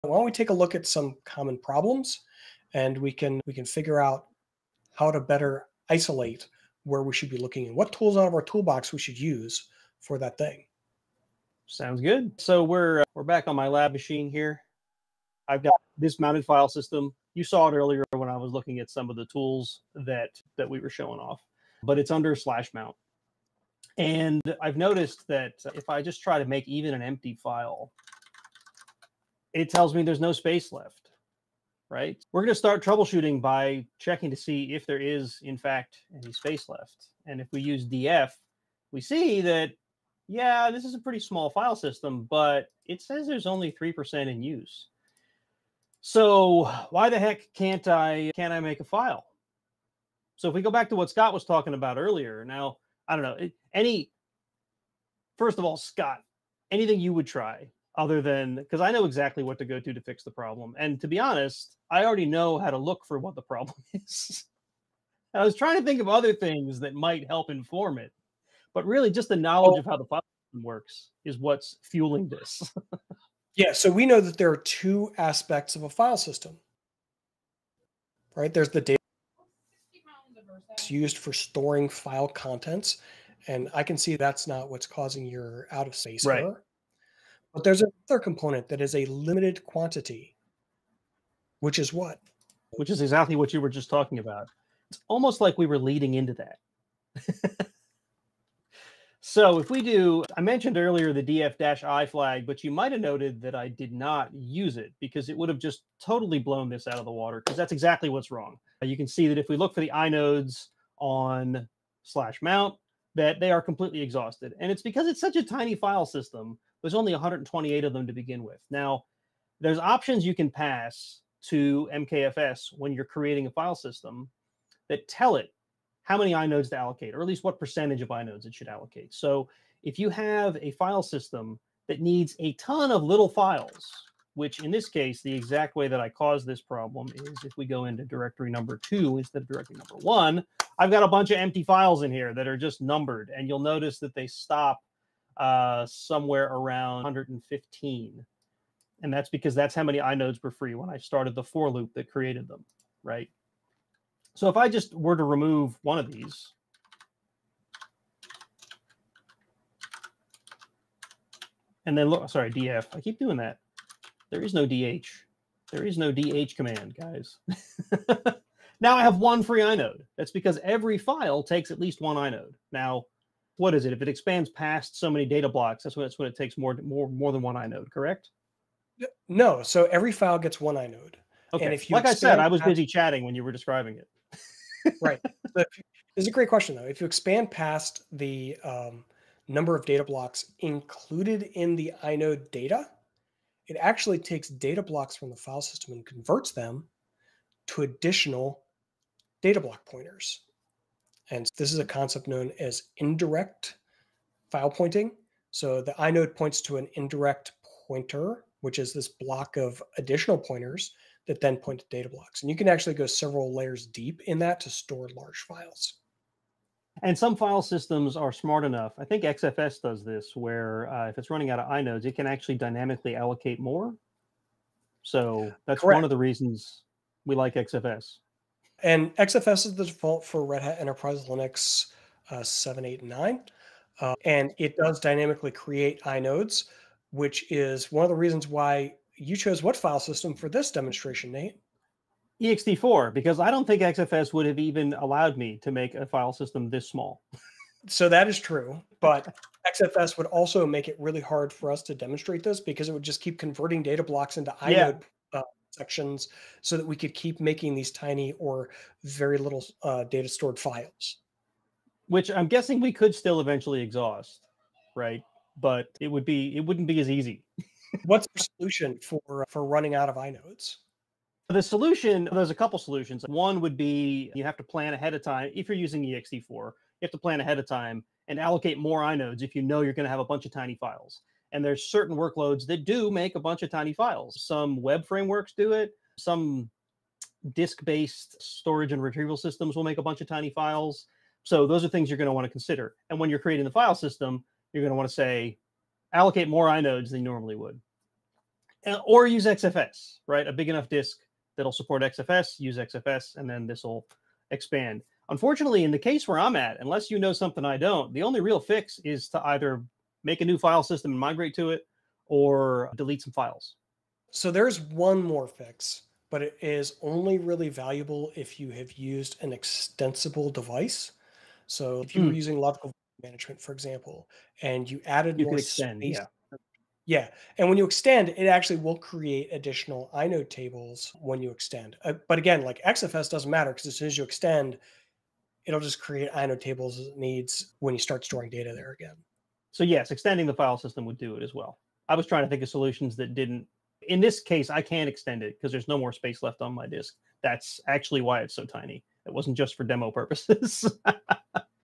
File System Out of Space? Learn How to Quickly Fix It
Join us as we troubleshoot file system errors. Follow along on a lab machine to understand how to diagnose and solve the 'no space left' issue despite seeing available space. We will explore the role of inodes, the limitations of small file systems, and solutions like using XFS or planning inode allocation. Learn practical steps to fix space issues by extending the file system or migrating to a new one.

00:00 Troubleshooting File System Issues
00:32 Initial Investigation: Checking Disk Space
01:13 Understanding Inodes and Their Role
01:39 Identifying the Problem: Exhausted Inodes
04:28 Solutions and Workarounds for Inode Exhaustion
05:05 Conclusion and Final Thoughts